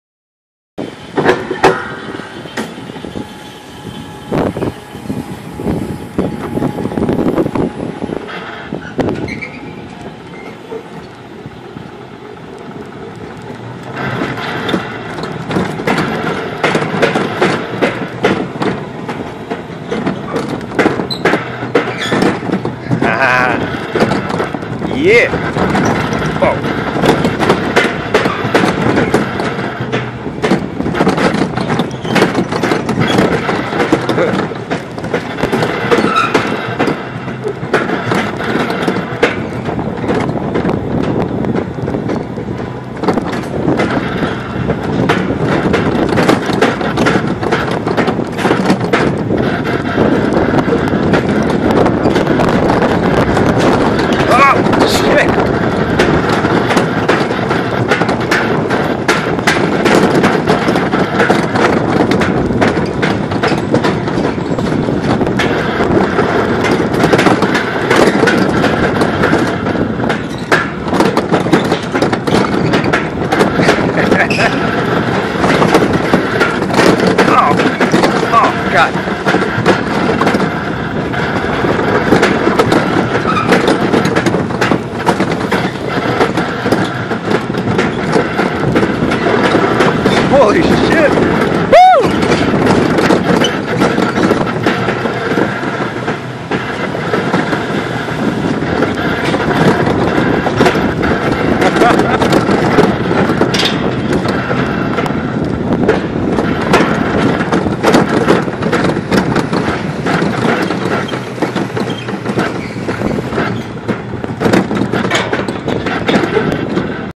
ALPINE COASTER at Attitash in New Hampshire!
Attitash has a bunch of things to do in the off season... This little roller coaster was so much fun! Up to 2 people can fit in a car at a time and the speed is controlled by levers on the side.